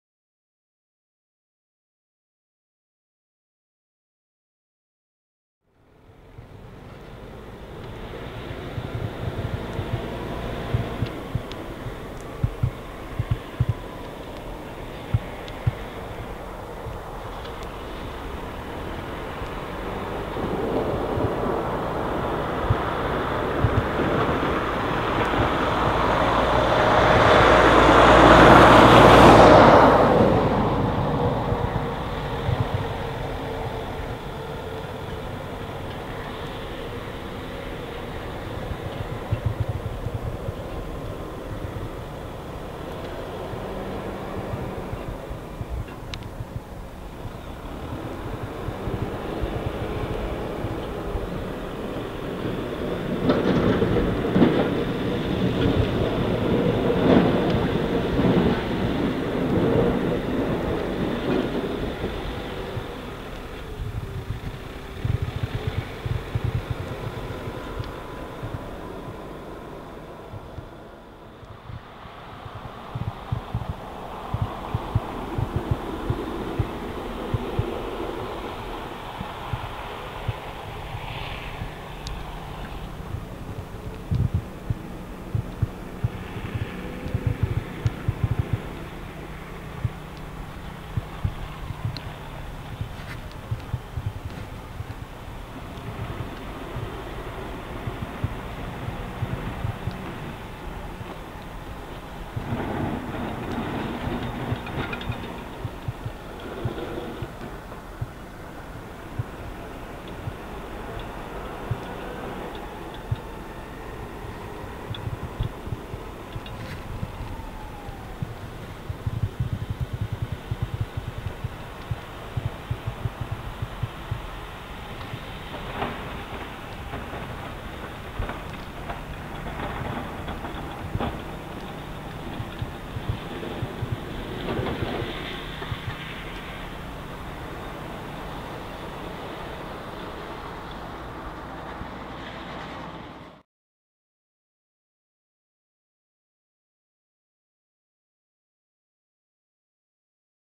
Volvo BM EL70 Plowing Snow
Volvo BM EL 70 Plows The Sidewalk.

Volvo BM EL 70 Plogar Trottoaren.